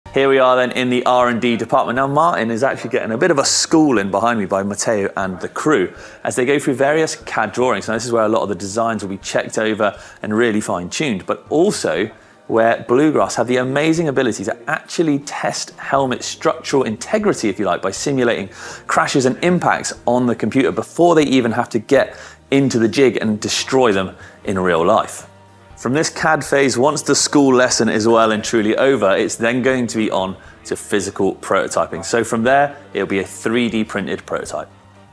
How MTB Helmets Are Designed & Prototyped! 🤔
Have you ever wondered how protection brands get their products from computers to trails? We've teamed up with Bluegrass to gain some insight into all the production stages that are involved in creating full-face lids.

🚲 Bluegrass

📺 Full Video LIVE NOW on our YouTube! 🎬

Useful Links:
Latest Offers In Our Shop!